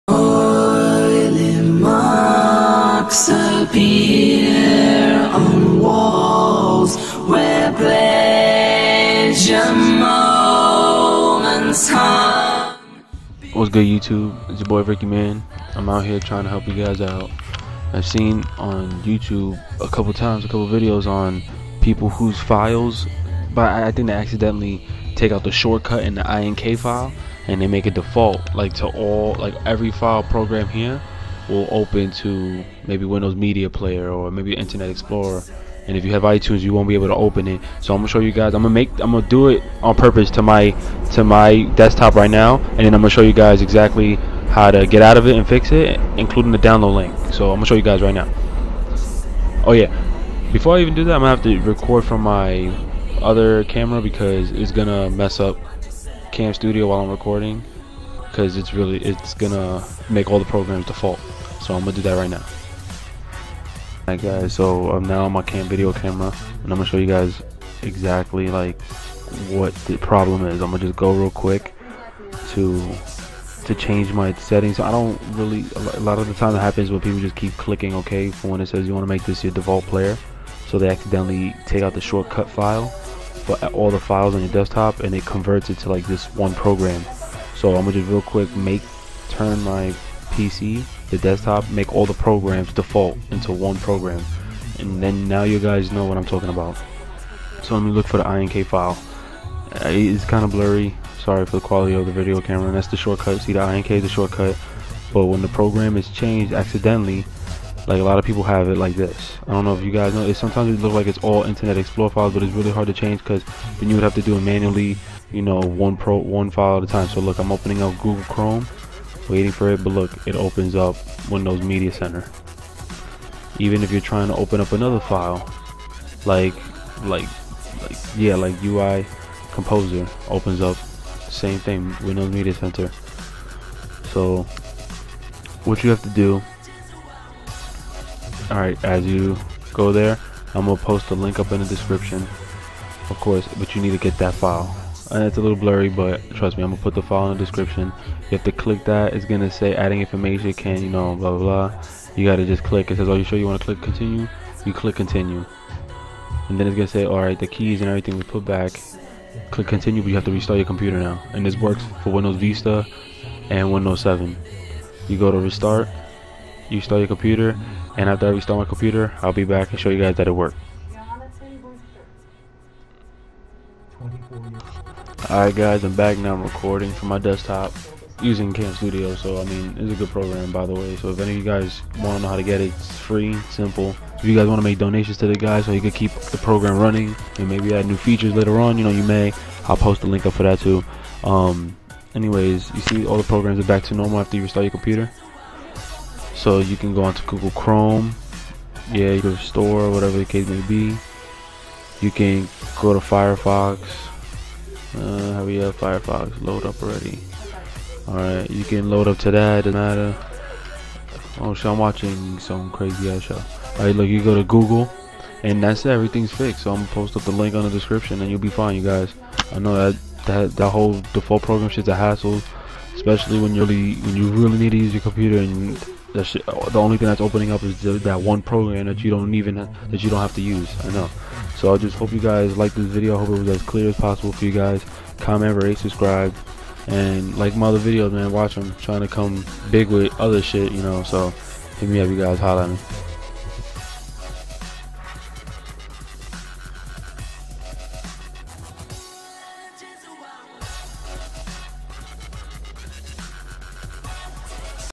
INK. FIle Error windows 7/ Vista FIXED
all you need to do is click that specific link  unrar it on ur desktop and double click it and click ok to everything it says then restart pc.. IF YOU HAVING TROUBLE GETTING ON WEBPAGE BECAUSE OF THE DEFAULT FIRES EASY TRICK I DO IS GO TO START THEN TYPE FIREFOX OR WHATEVER WEB BROWSER U HAVE AND CLICK THE SETUP FOR THAT BROWSER AND AFTER IT INSTALLS AGAIN (WILL NOT DELETE BOOKMARKS OR HISTORY DON'T WORRY) IT WILL AUTOMATICALLY OPEN THE WEB BROWSER AND DO EXACTLY WHAT I SAID =) ANY ISSUES PLEASE COMMENT, I HOPE THIS HELPS